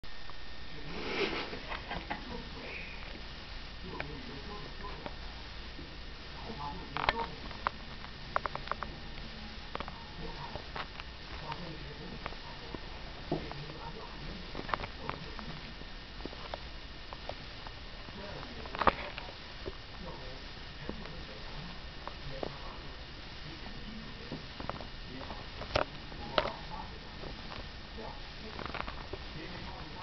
Cute Bunnies
Cute bunnies playing with each other